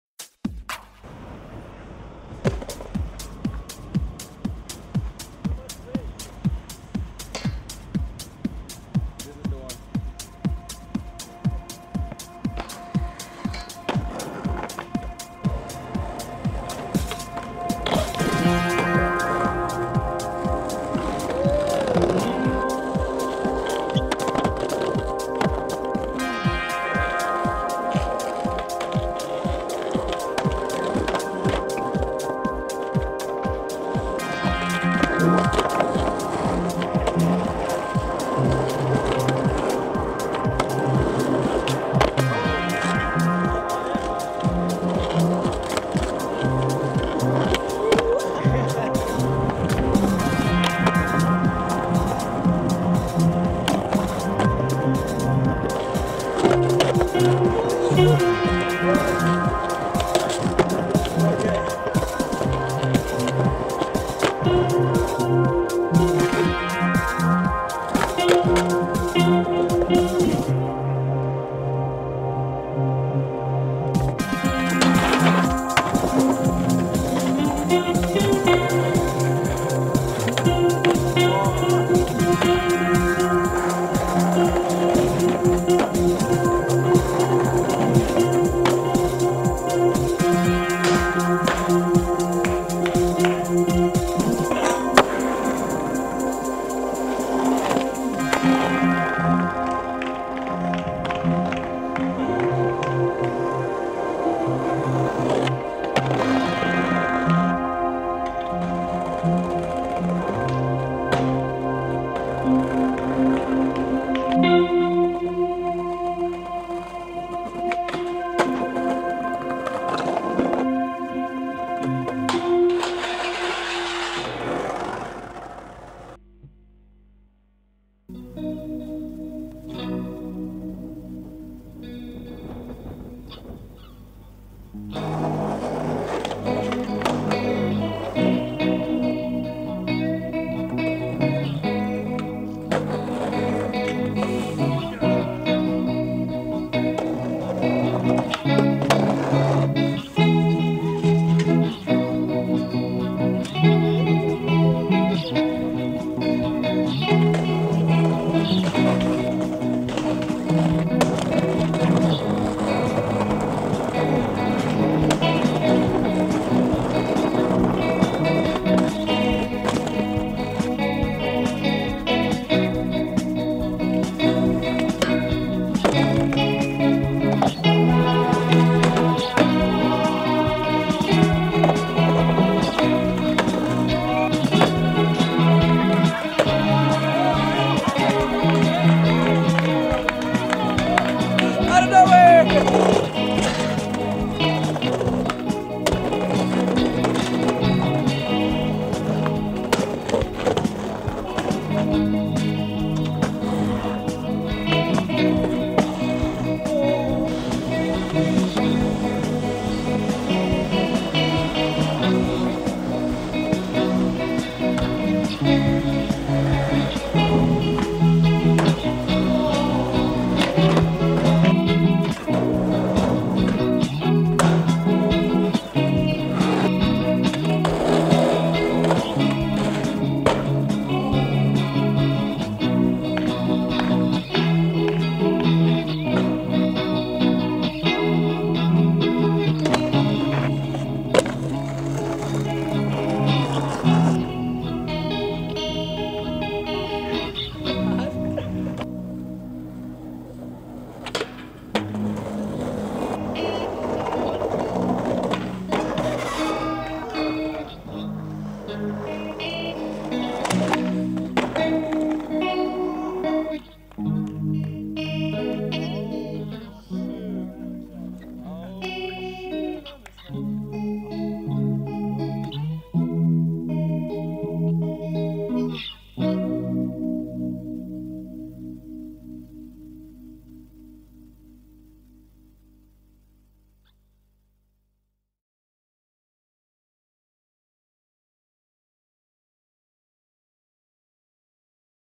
Place Presents: Malmö
We proudly present to you this film by Leon Rudolph feat.: Jugga, Sondre & Amandus, Ville Wester, Elias Mensi, Samuel Norgren, John Dahlquist, Santiago Sasson, Tom Botwid, Koffe Hallgren & Sarah Meurle.